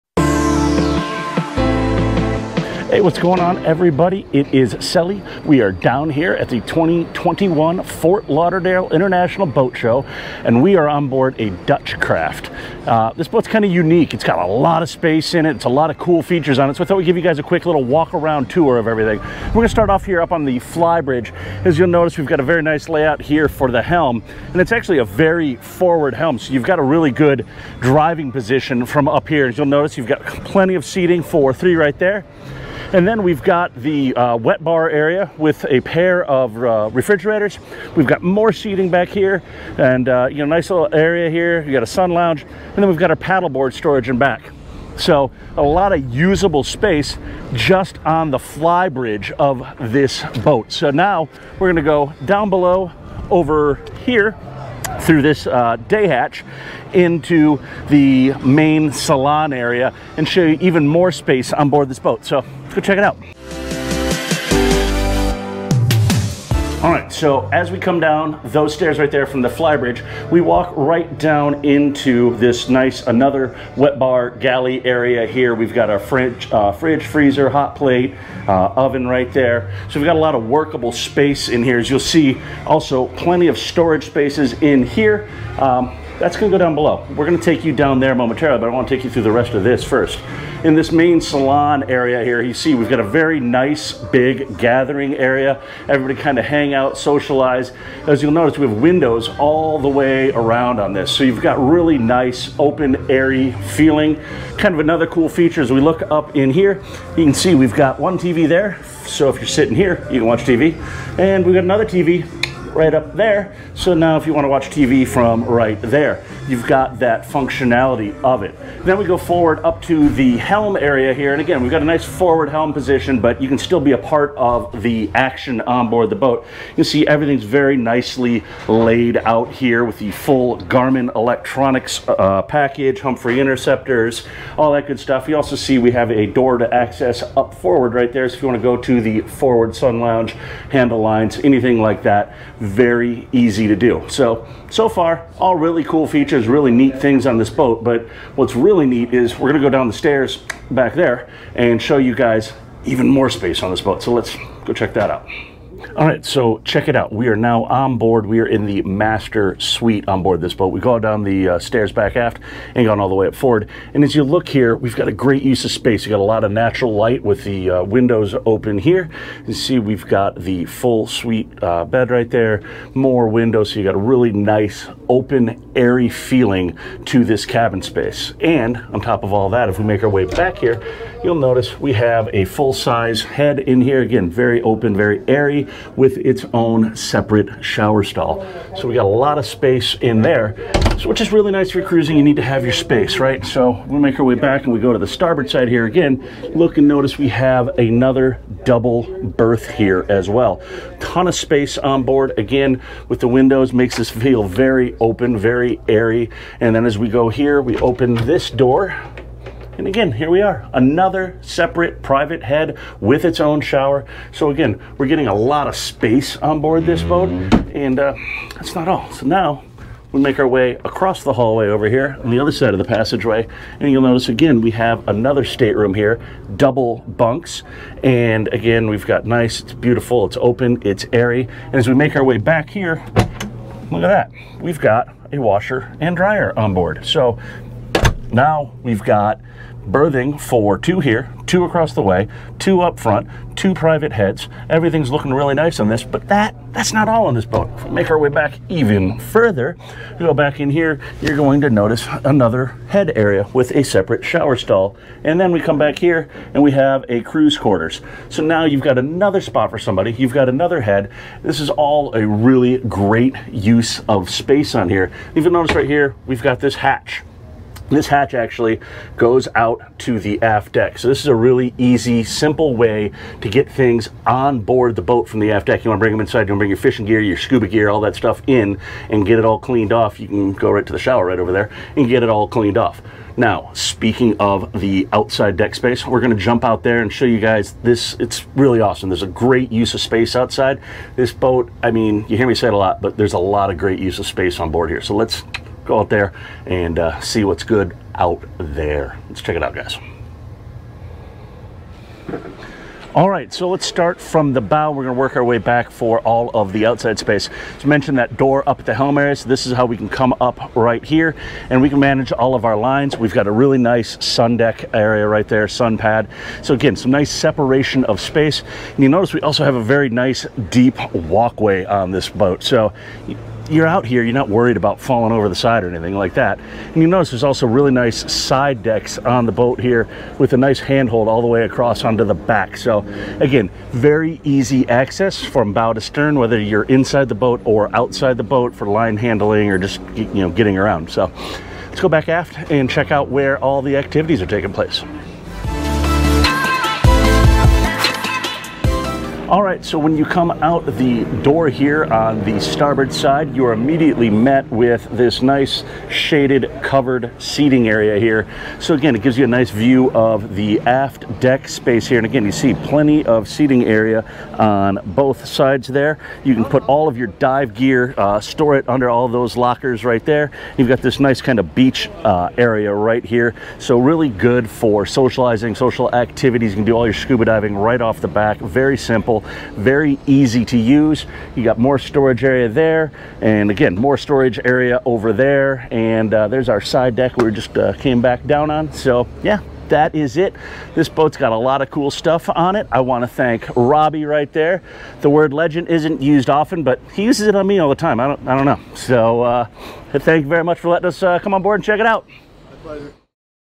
Dutch Craft 54 walk thru
Something a little different this time.  While visiting FLIBS 2021, this boat caught our eye so we thought we would give you a walk through of this special boat.  Check it out and let us know what you think.  She is available for charter check out for more details.

We are down here at the 2021 Fort Lauderdale International Boat Show and we are on board a Dutch craft. This boat kind of unique. It's got a lot of space in it. It's a lot of cool features on it. So I thought we give you guys a quick little walk around tour of everything. I'm going to start off here up on the Flybridge. As you'll notice, we've got a very nice layout here for the helm, and it's actually a very forward helm. So you've got a really good driving position from up here and you'll notice you've got plenty of seating for three right there. And then we've got the wet bar area with a pair of refrigerators. We've got more seating back here and a nice little area here. We've got a sun lounge and then we've got a paddle board storage in back. So a lot of usable space just on the fly bridge of this boat. So now we're going to go down below over here through this day hatch into the main salon area and show you even more space on board this boat. So check it out. All right. So as we come down those stairs right there from the Flybridge, we walk right down into this nice another wet bar galley area here. We've got our fridge, freezer, hot plate oven right there. So we've got a lot of workable space in here, as you'll see. Also plenty of storage spaces in here that's going to go down below. We're going to take you down there momentarily. But I want to take you through the rest of this first. In this main salon area here. You see we've got a very nice big gathering area. Everybody kind of hang out, socialize, as you'll notice, with windows all the way around on there. So you've got really nice, open, airy feeling, kind of another cool feature as we look up in here, you can see we've got one TV there. So if you're sitting here, you can watch TV and we've got another TV right up there. So now if you want to watch TV from right there, you've got that functionality of it. And then we go forward up to the helm area here. And again, we've got a nice forward held position, but you can still be a part of the action on board the boat. You see, everything's very nicely laid out here with the full Garmin Electronics package, Humphree interceptors, all that good stuff. We also see we have a door to access up forward right there. If you want to go to the forward Sun Lounge, handle lines, anything like that, very easy to do. So so far all really cool features, really neat things on this boat. But what's really neat is we're going to go down the stairs back there and show you guys even more space on the spot. So let's go check that out. So check it out. We are now on board. We are in the master suite on board this boat. We go down the stairs, back aft and go on all the way up forward. And as you look here, we've got a great use of space. You get a lot of natural light with the windows open here. You see, we've got the full suite bed right there, more windows. So you've got a really nice, open, airy feeling to this cabin space. And on top of all that, if we make our way back here, you'll notice we have a full sized head in here. Again, very open, very airy, with its own separate shower stall. So we've got a lot of space in there, which is really nice for cruising. You need to have your space, right? So we make our way back and we go to the starboard side here again, look and notice we have another double berth here as well. Ton of space on board again with the windows. Makes us feel very open, very airy. And then as we go here, we open this door. And again, here we are, another separate private head with its own shower. So again, we're getting a lot of space on board this boat, and that's not all. So now we make our way across the hallway over here on the other side of the passageway. You'll notice again, we have another state room here, double bunks. And again, we've got nice it's beautiful to open, it's airy. And as we make our way back here. Look at that, we've got a washer and dryer on board. So now we've got berthing for two here, two across the way to up front to private heads. Everything's looking really nice on this. But that that's not all on this boat. Make our way back even further. You go back in here, you're going to notice another head area with a separate shower stall. And then we come back here and we have a crew's quarters. So now you've got another spot for somebody, you've got another head. This is all a really great use of space on here. Even on this right here, we've got this hatch. This hatch actually goes out to the aft deck. So this is a really easy, simple way to get things on board the boat from the aft deck to bring them inside and bring your fishing gear, your scuba gear and get it all cleaned off.